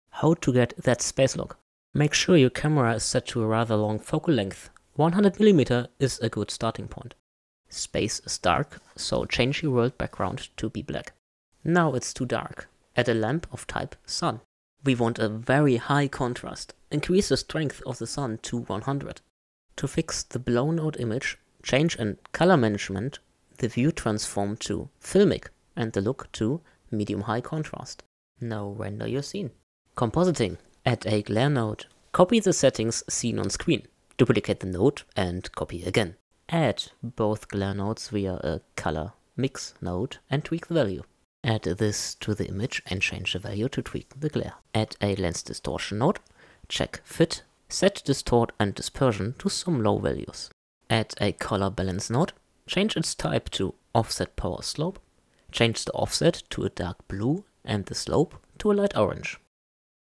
How to get that space look - 60 second Blender tutorial
A quick run down on how i light up and composite (super reduced node setup though) my space renderings.

Please use "medium high contrast"- I did say it right but captured it incorrectly in the video, sorry for that!

Make sure your albedo values of your surfaces are roughly where they should be (white paper has a value of ~0.65, there should not be a lot in you scene which is brighter), otherwise your result will be a completely white image.

I am somtimes on Twitter